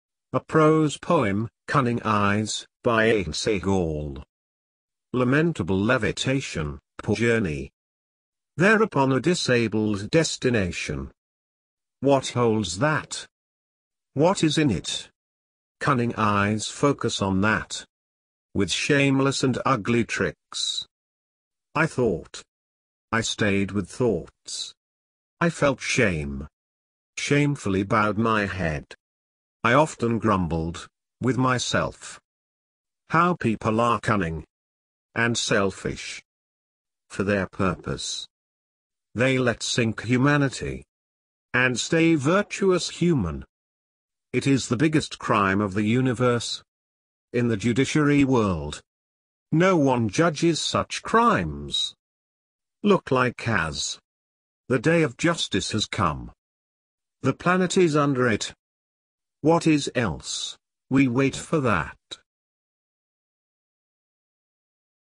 Cunning Eyes --- by Ehsan Sehgal
A prose poem --- Cunning Eyes --- by famous poet Ehsan Sehgal

Cunning Eyes
Lamentable-levitation, poor journey
Thereupon a disabled-destination
What holds that?
What is in it?
Cunning eyes focus on that
With shameless and ugly tricks
I thought
I stayed with thoughts
I felt shame
Shamefully bowed my head
I often grumbled, with myself
How people are cunning
And selfish
For their purpose
They let sink humanity
And stay virtuous human
It is the biggest crime of the universe
In the judiciary world,
No one judges such crimes
Look like as
The day of justice has come
The planet is under it
What is else, we wait for that?
Ehsan Sehgal